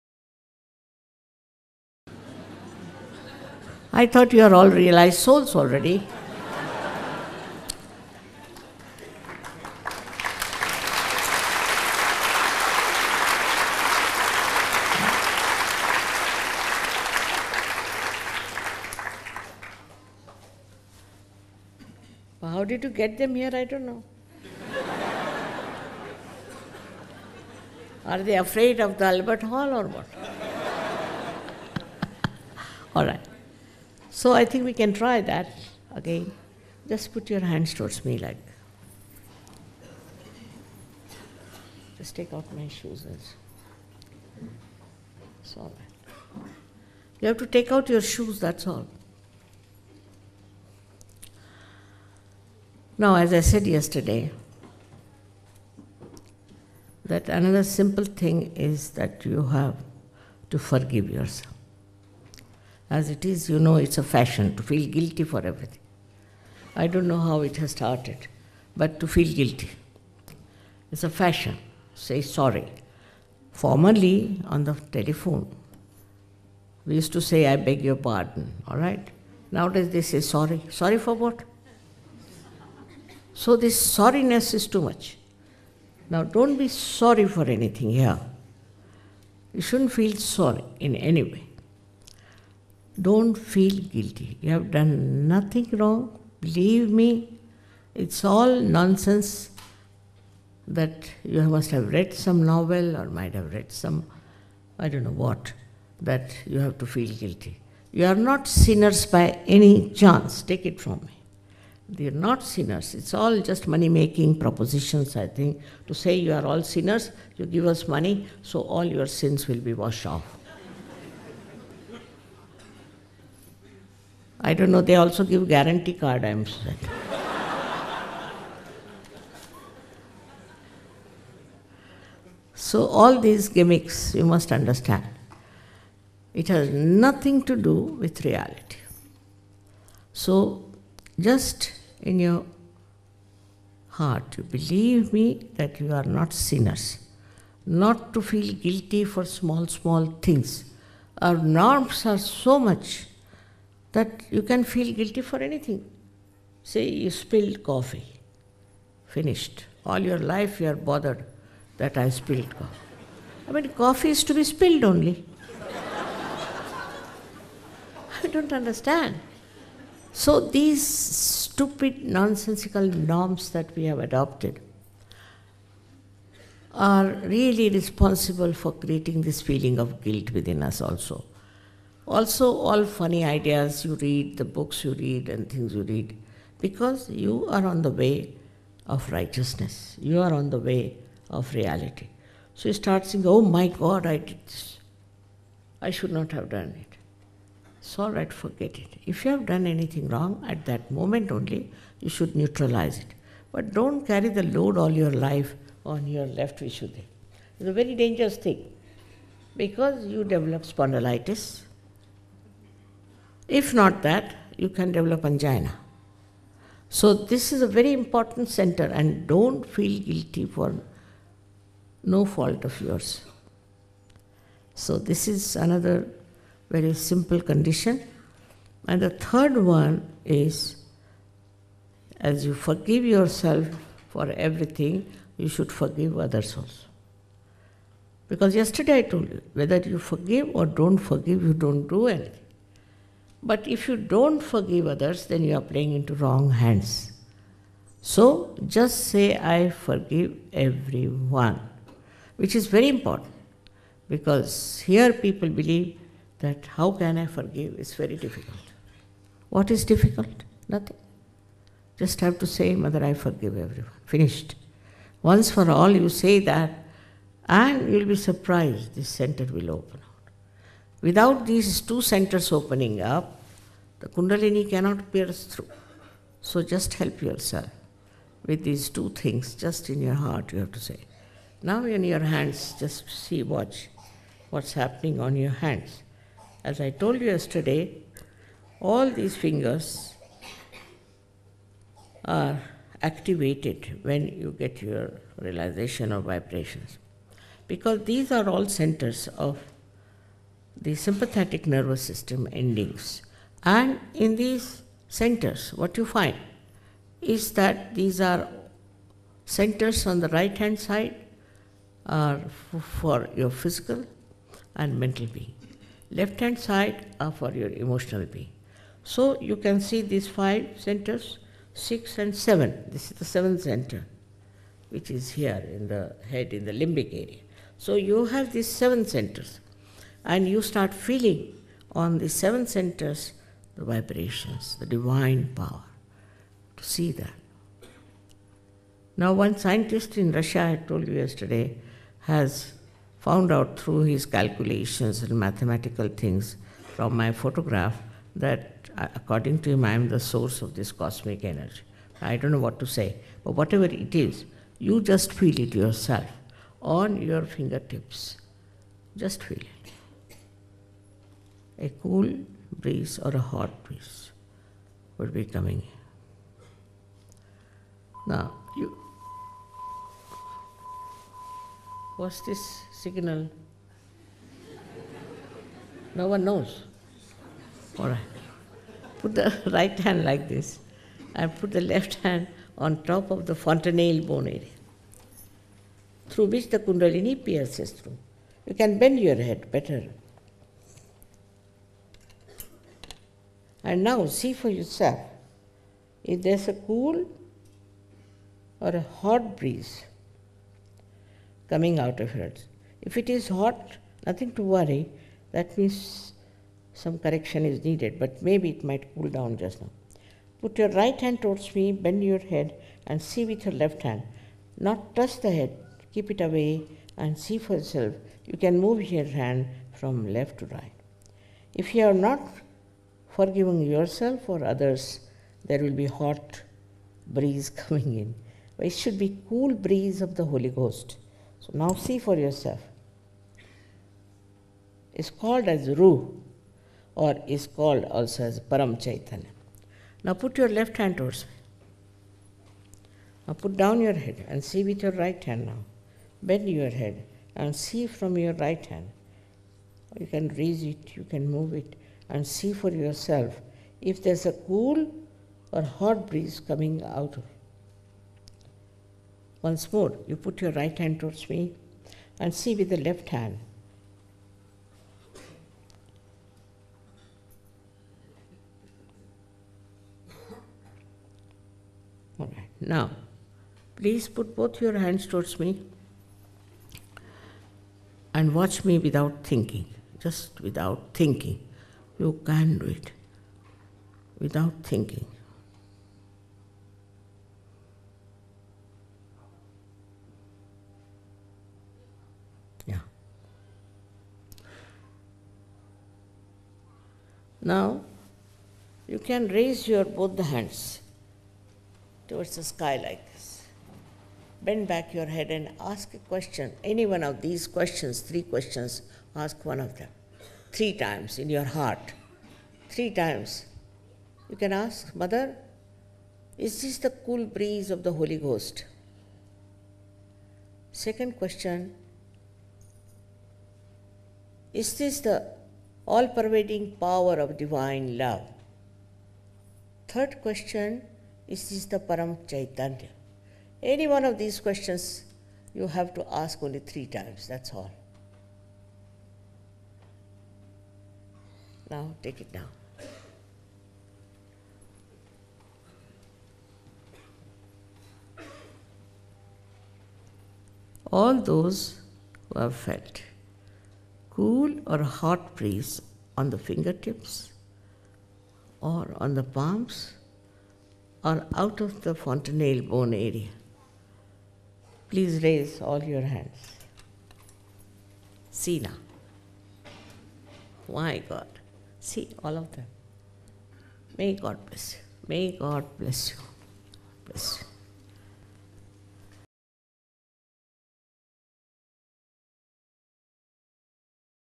1998 July 6 - Don't Feel Guilty - London - Talk by Shri Mataji - 9m44
Space bef / aft 2 sec, 5 sec for playing in video player like VLC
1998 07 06 ForgiveYourself London w Realization spBA2 5 9m44